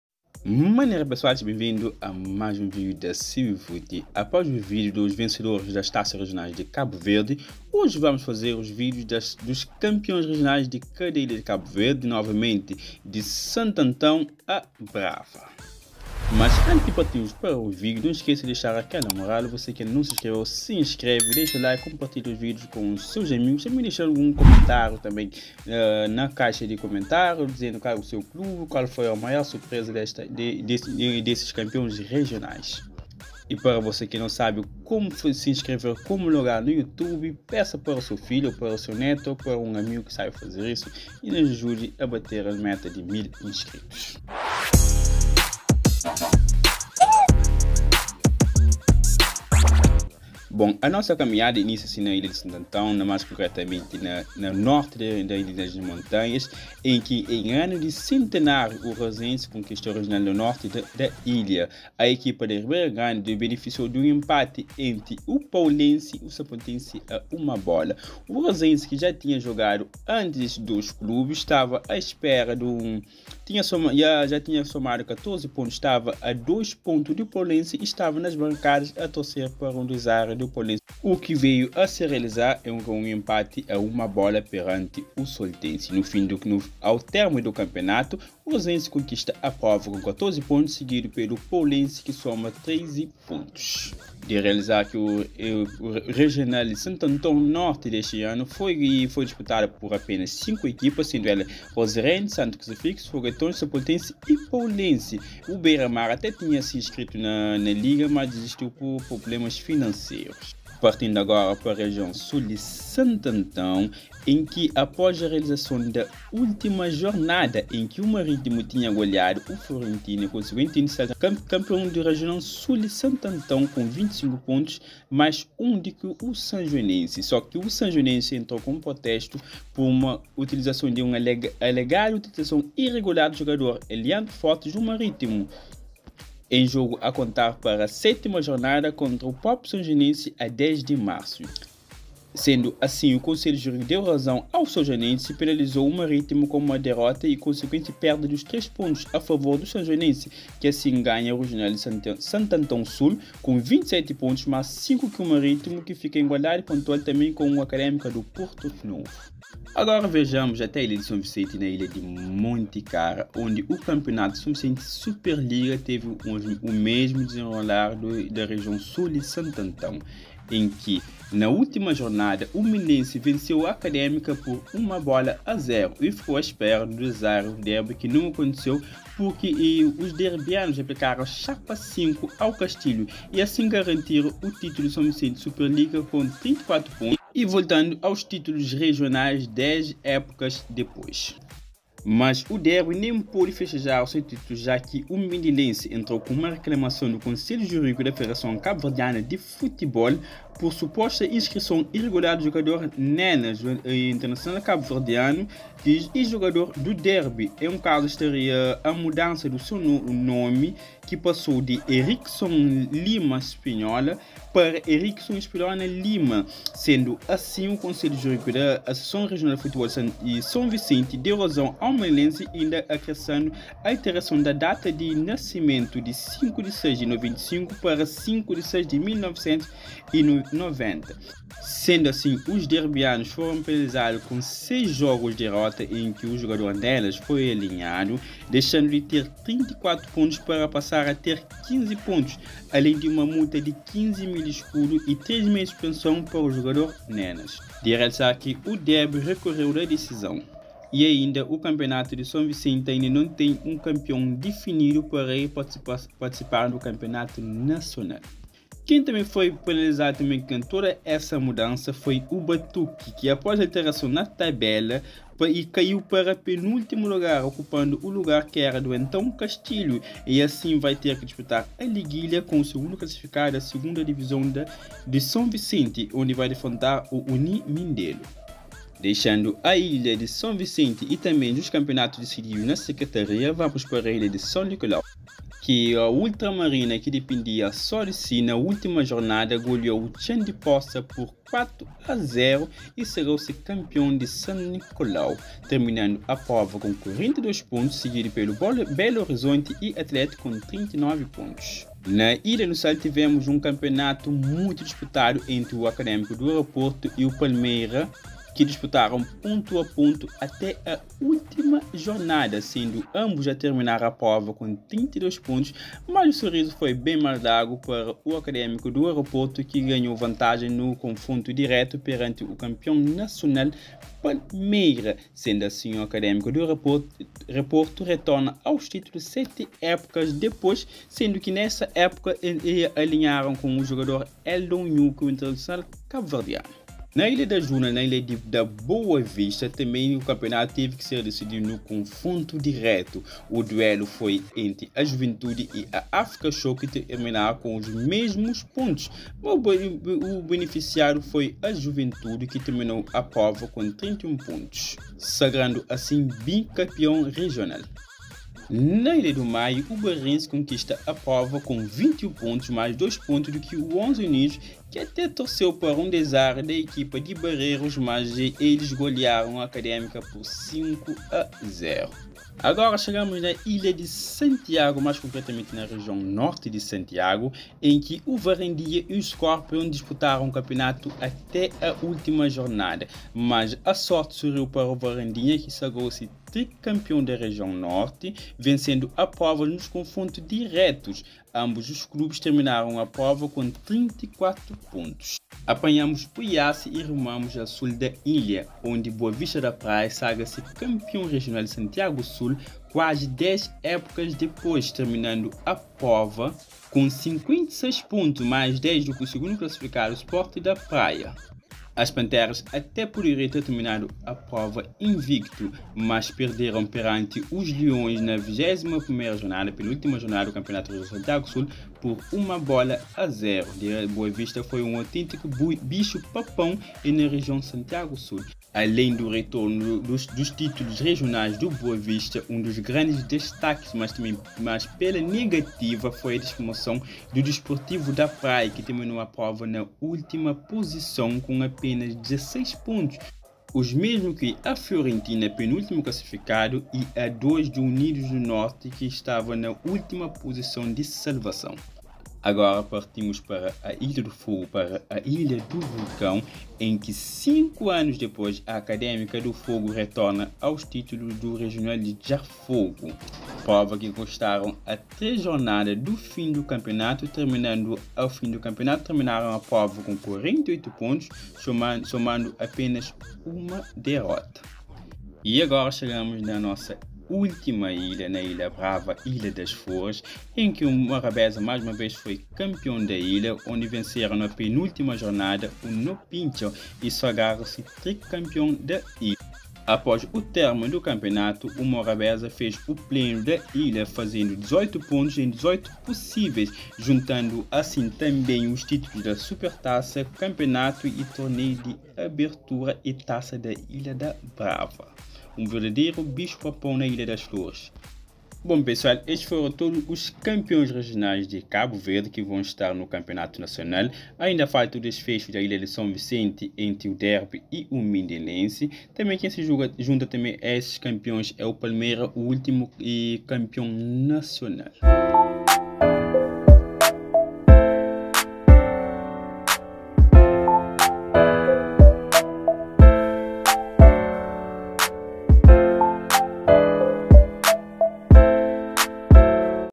CAMPEÕES REGIONAIS DE CABO VERDE 23/24
saiba quem são os vencedores das ligas regionais de cabo verde época 2023/2024
e as desenrolar das desições  das secretarias da Soncent Super liga.